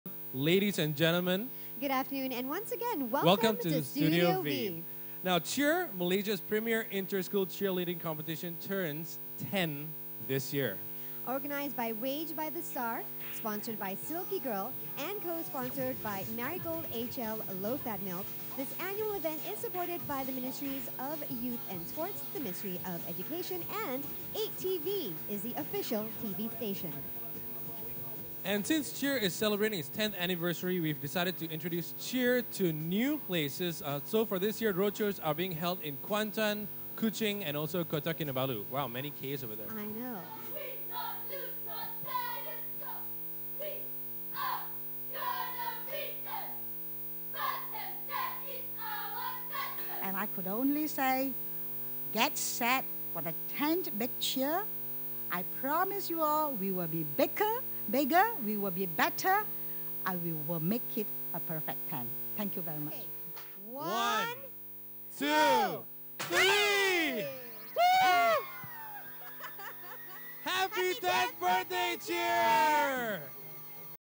Cheer 2009 launch
The Star officially launches the 10th annual national level cheerleading event at Studio V at 1 Utama Shopping Centre in Petaling Jaya.